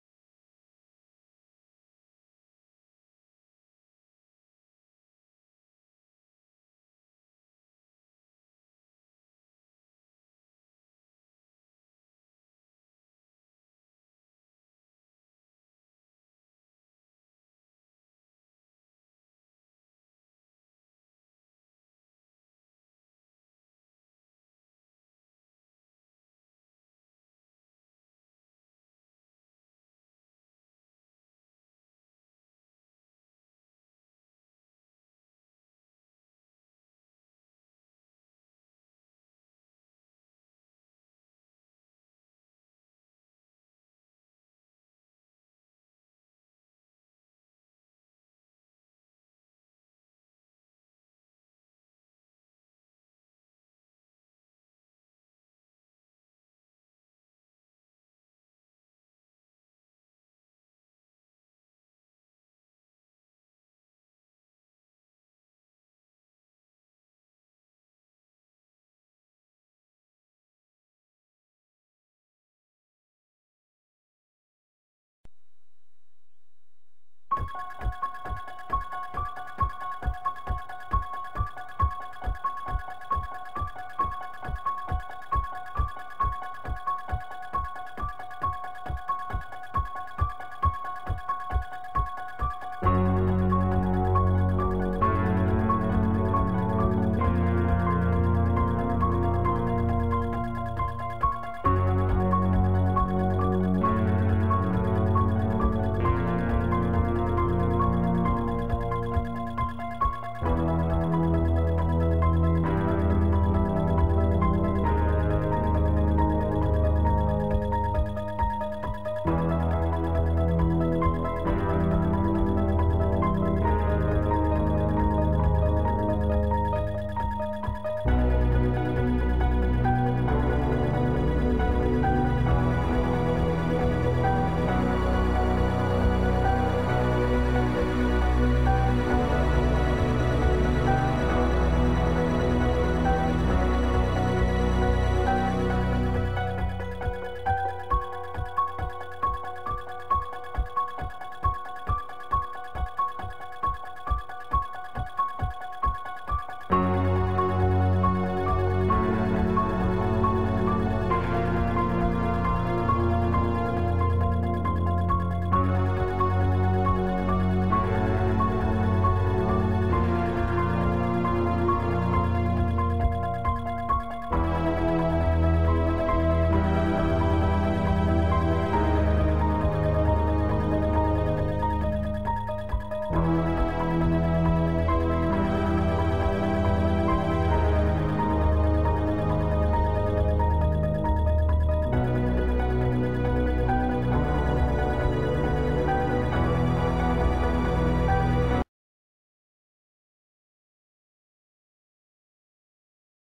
Re re re: Ghosts Spirits (movie) In Kidscom
lol, this time its more funny! cuz of the pics of grandma, lol, but its fake, its actually funny to me, but also scary! enjoy!